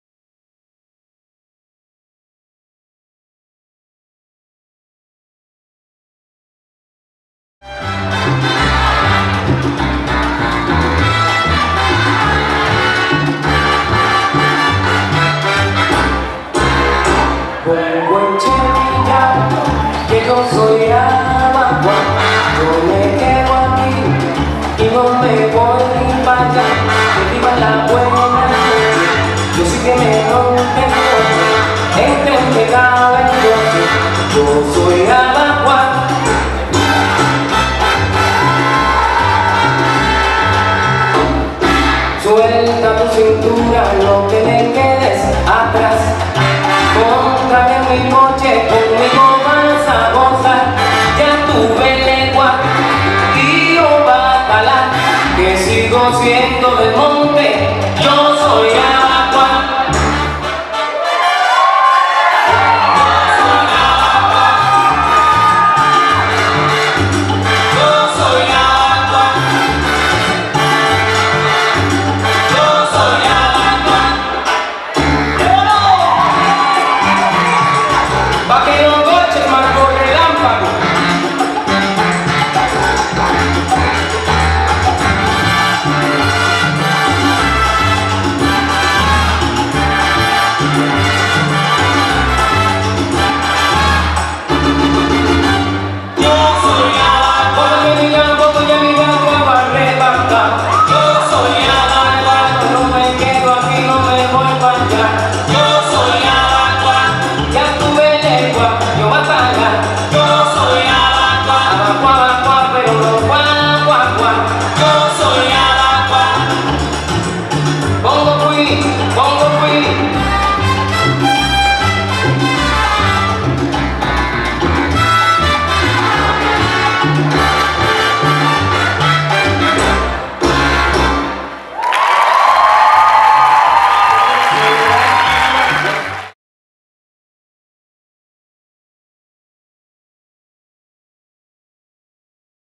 On2ourage
On2ourage at the Rose City Salsa and Timba Festival.